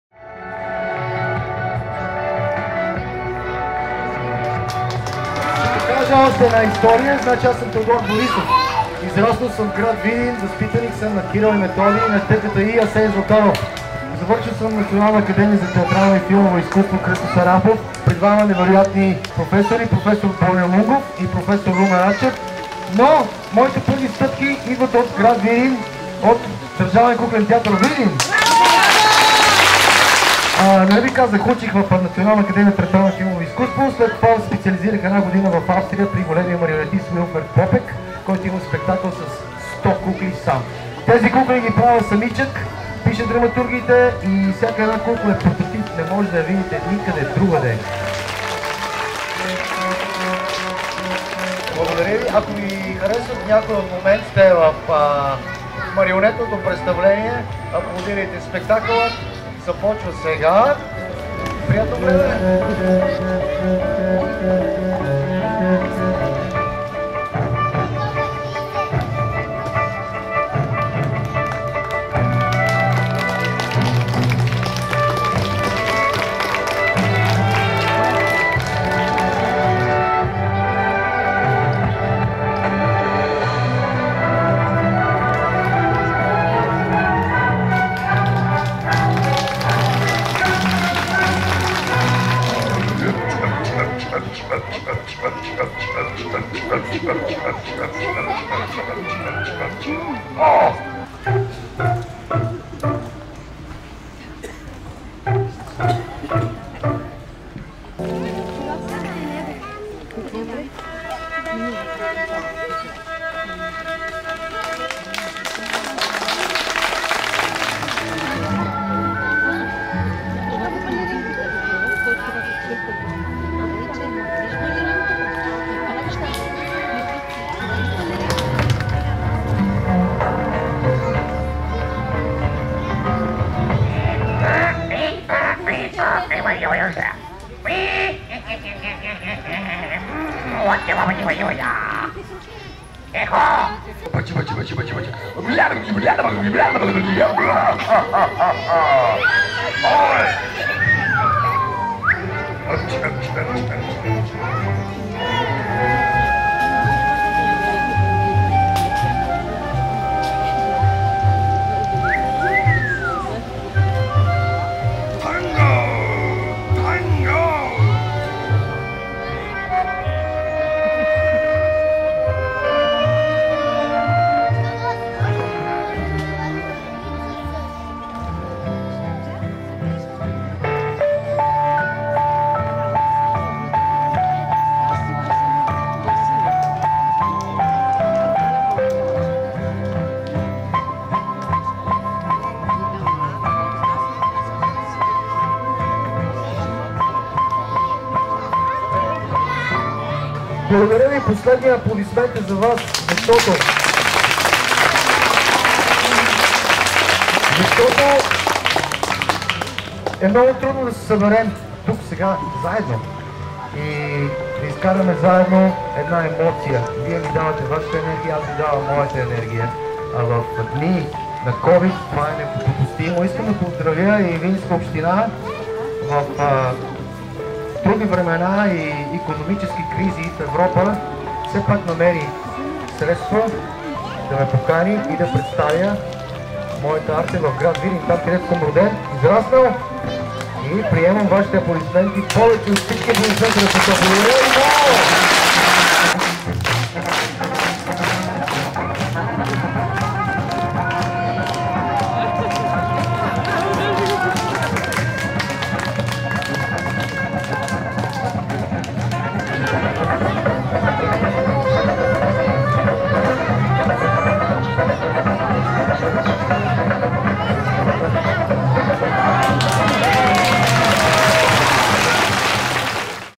BTA: "Светът на марионетките на Теодор Борисов" гостува във Видин
Представлението "Светът на марионетките" гостува във Видин - родния град на автора на спектакъла и на куклите Теодор Борисов. В рамките на уникалното авторско шоу бяха показани осем микро драматургии, подходящи както за деца, така и за възрастни. Пиесите, както и марионетките, с помощта на които те бяха разказани, са дело на Теодор Борисов. Той всяка година изработва нови персонажи и сменя жанра на представлението, така че спектаклите му винаги са нови, различни и предизвикват любопитство у публиката.

След края на представлението Теодор Борисов благодари на всички, които са помогнали спектакълът да се реализира в родния му град. "Много е трудно в днешно време да се съберем и заедно да преживеем една емоция. Вие ми давате вашата енергия, а аз - моята, но в дни на COVID това не е лесно осъществимо. Затова искам да поздравя Видинска община, че в тези трудни времена ми предостави възможност да покажа театъра, който правя, в града, където съм израснал“, каза артистът. Той сподели, че първите си стъпки в кукленото изкуство е направил в Държавен куклен театър - Видин като стажант-актьор и благодари на директора на театъра Павлина Трифонова, че е била неговият пръв педагог.

Според автора в кукления и по-специално в марионетния театър трябва да се изпипват нещата до последната подробност, да има детайл, да има занаятчийство в изработката. По думите му неговите кукли са уникални и новаторски. Сред тях има такива, на които дори клепачите им се повдигат, толкова прецизна е изработката, други пък мърдат пръстите си, устните и дори носа. Има кукла, която държи бастун в ръка и, подпряна на него, се разхожда по сцената, както и такава, която повдига чаша и пие вода. Сред уникалните му кукли е и марионетка, която свири на истинска цигулка.

Авторът на спектакъла Теодор Борисов е  роден, учил и израстнал във Видин. Завършил е НАТФИЗ при  проф. Боньо Лунгов и проф. Румен Рачев, след което е специализира в Австрия. От близо 20 години живее и твори в Милано. Той е преподавател и доцент в Болонския университет "Alma Mater Studiorum". Преподавал е също така и в Етиопия и Тунис. Той е организатор и на първия Циркус Фестивал за Африкански трупи. В творческата си биография от 2003 година до днес има над 4000 спектакъла.

На финала на представлението "Светът на марионетките" Теодор Борисов даде възможност на видинските деца да се докоснат до уникалните кукли и дори да ги водят.

Репортер и оператор - Ирена Данаилова
Монтаж - Валя Ковачева